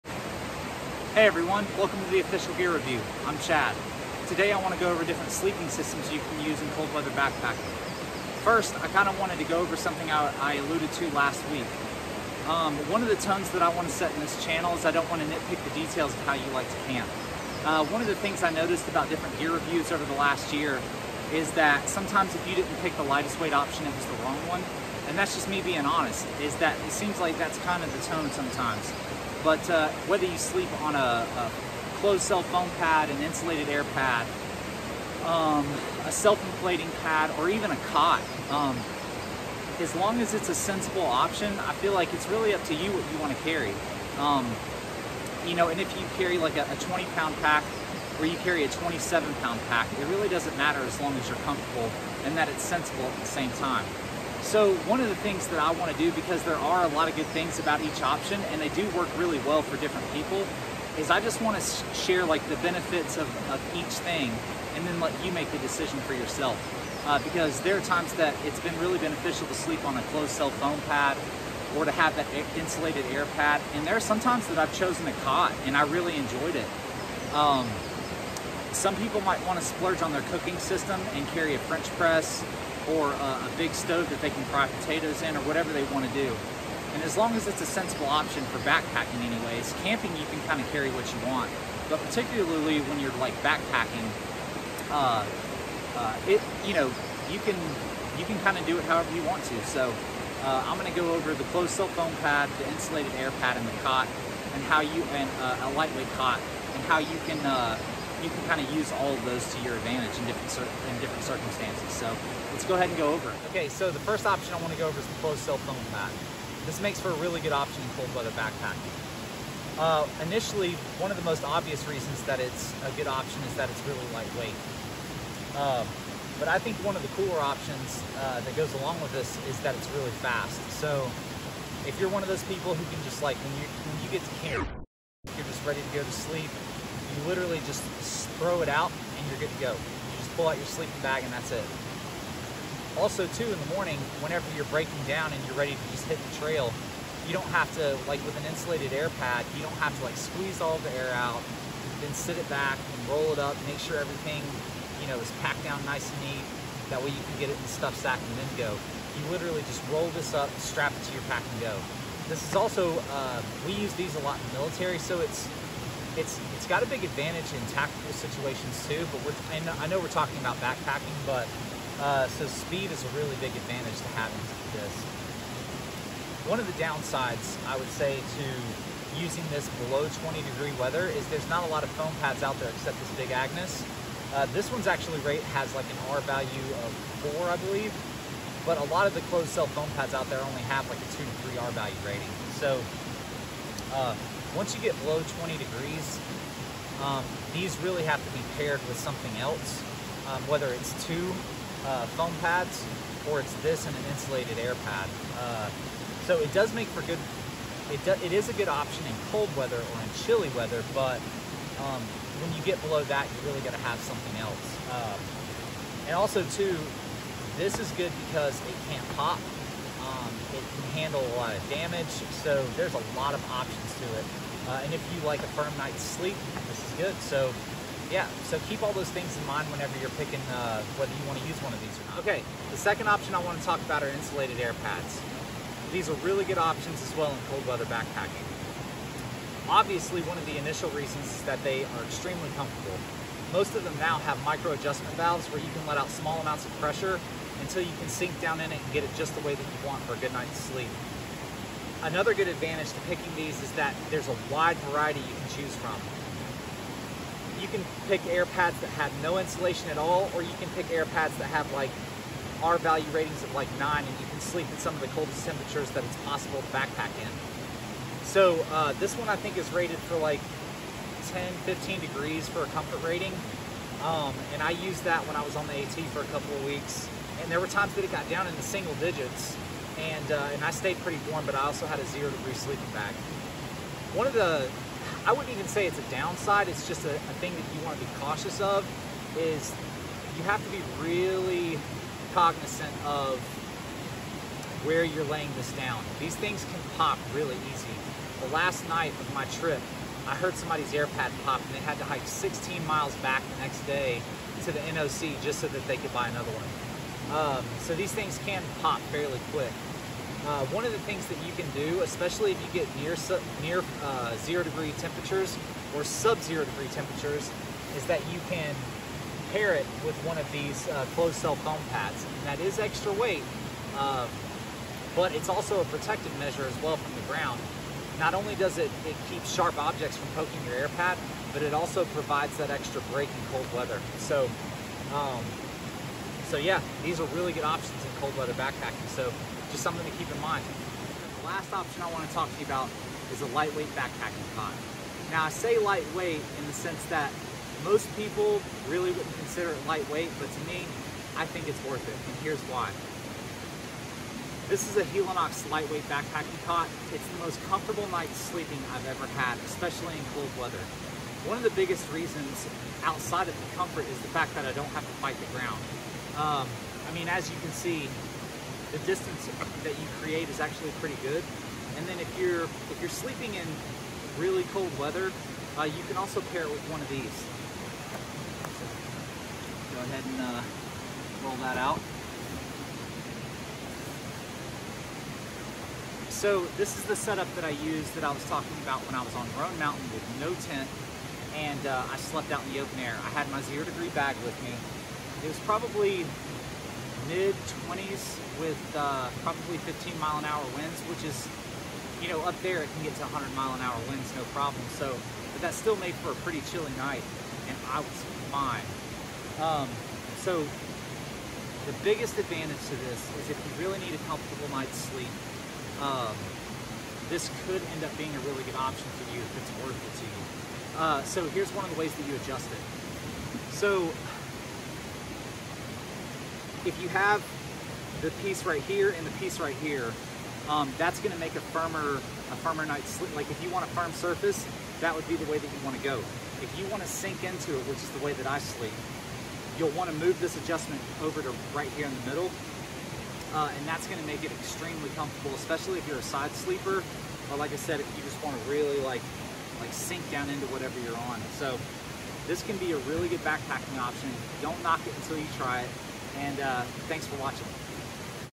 Backpacking cold weather sleep systems
There are a couple of really good options to choose from to get a good nights sleep camping in cold weather. Along with the popular options typically chosen for cold weather sleeps systems, there's another method I use which I talk about towards the end of the video (it's one of my favorites). Also about the intro. I am in the process of slimming down my pack to sub 25lbs with a 4 day supply. I am exploring the ultra light game a lot more because of things I'm finding appealing about it. One thing I've noticed on a few backing packing channels (popular ones), is a strong push for the lightest option always being the best. Sometimes if the gear is heavier for cost or comfort reasons, (some people just can't do closed cell foam with bad backs) and you have to carry a few extra pounds, that's ok! The heart of getting outdoors is being in nature to relax and have a blast! That's why I want to show people there are some really comfortable options out there that may be worth it. Ultralighting is legit (in the process of converting myself)! In cold weather though, don't be miserable if it means a 24lb pack vs. 21lb's. Wait till it warms up. You're better off sleeping well! Check it out, and enjoy!